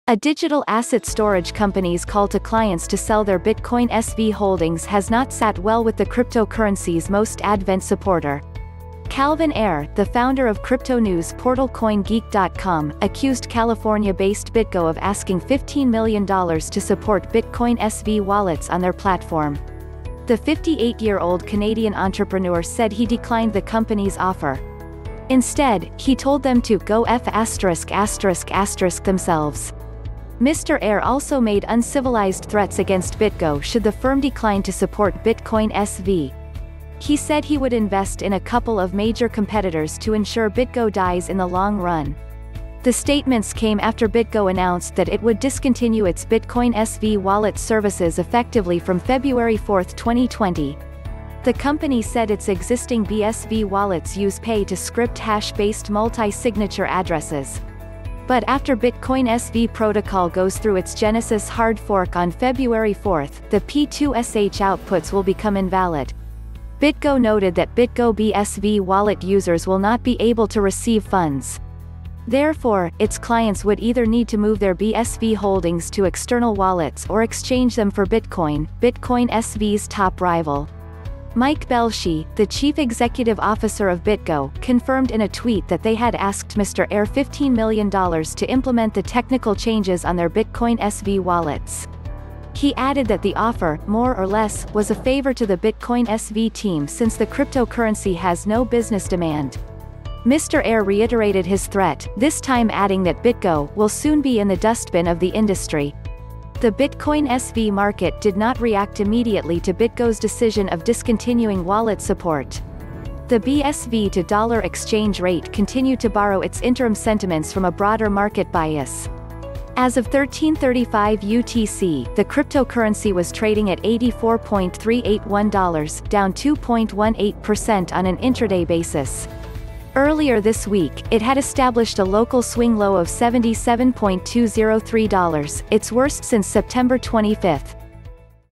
BitGo Asked for $15M to Support Bitcoin SV Cryptocurrency Ayre
Everyone should see it!

A digital asset storage company’s call to clients to sell their Bitcoin SV holdings has not sat well with the cryptocurrency’s most advent supporter. Calvin Ayre, the founder of crypto news portal CoinGeek.com, accused California-based BitGo of asking $15 million to support Bitcoin SV wallets on their platform. The 58-year old Canadian entrepreneur said he declined the company’s offer. Instead, he told them to “go f*** themselves.”Mr. Ayre also made uncivilized threats against BitGo should the firm decline to support Bitcoin SV. He said he would invest in a couple of major competitors to ensure BitGo dies in the long-run. The statements came after BitGo announced that it would discontinue its Bitcoin SV wallet services effectively from February 4, 2020. The company said its existing BSV wallets use Pay-to-Script-Hash-based multi-signature addresses. But after Bitcoin SV protocol goes through its Genesis Hard Fork on February 4, the P2SH outputs will become invalid. BitGo noted that BitGo BSV wallet users will not be able to receive funds. Therefore, its clients would either need to move their BSV holdings to external wallets or exchange them for bitcoin – Bitcoin SV’s top rival. Mike Belshe, the chief executive officer of BitGo, confirmed in a tweet that they had asked Mr. Ayre $15 million to implement the technical changes on their Bitcoin SV wallets. He added that the offer – more or less – was a favor to the Bitcoin SV team since the cryptocurrency has no business demand. Mr. Ayre reiterated his threat, this time adding that BitGo “will soon be in the dustbin of the industry.”The Bitcoin SV market did not react immediately to BitGo’s decision of discontinuing wallet support. The BSV-to-dollar exchange rate continued to borrow its interim sentiments from a broader market bias. As of 1335 UTC, the cryptocurrency was trading at $84.381, down 2.18 percent on an intraday basis. Earlier this week, it had established a local swing low of $77.203, its worst since September 25.